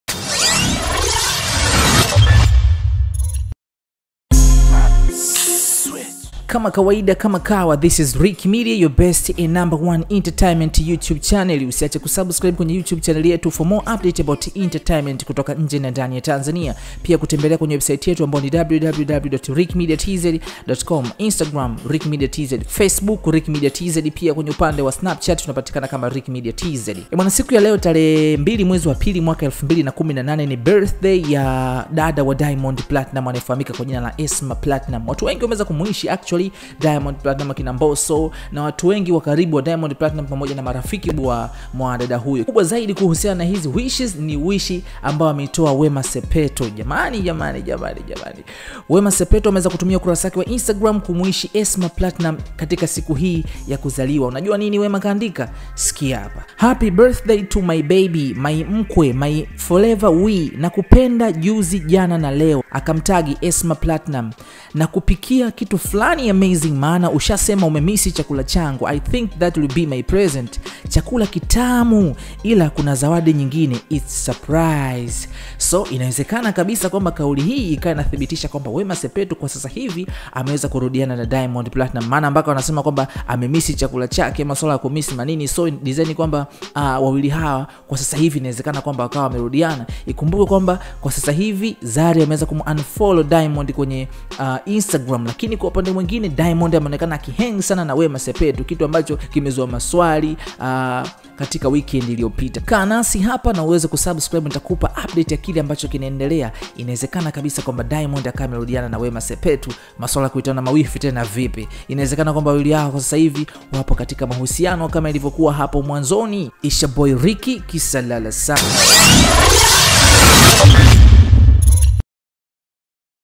WEMA SEPETU Ajipendekeza kwa DADA wa DIAMOND, Ona Alichoandika LEO
Leo ni Birthday ya Esma ambaye ni dada wa Diamond Platnum. Wema Sepetu hajataka kuwa nyuma ameamua kuandika ujumbe huu Instagram.
RICK MEDIA tupo pamoja nawe kukupatia habari za Burudani na Michezo ambazo zinajitokeza kila siku pande zote za dunia.